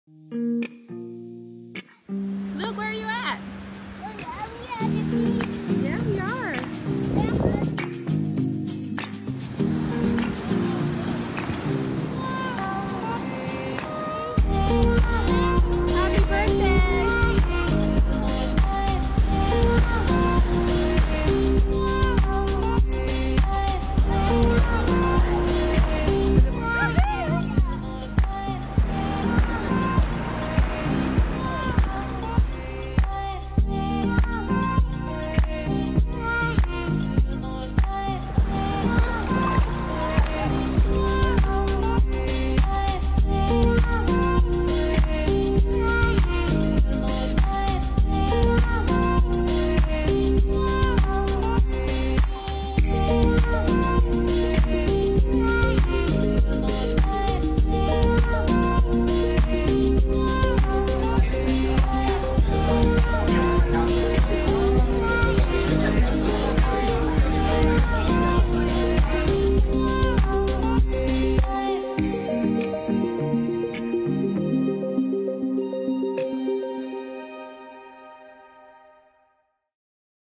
Family at San Diego Beach
Playing in the sand and having fun at the beach